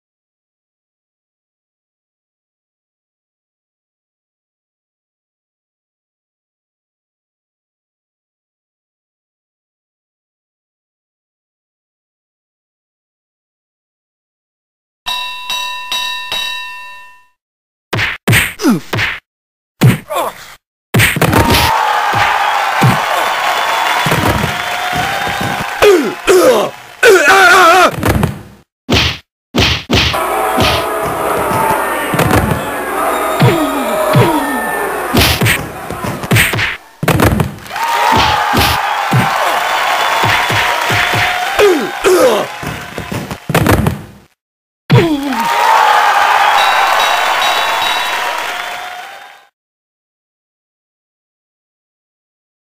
MMA Superfight 1: Mir VS Silva
Anderson SIlva VS Frank Mir, reuploaded with the new MMA Superfight Title on the line.

The MMA Superfight Championship can be defended in any promotion (UFC, PRIDE) and under any rules, in an open weight class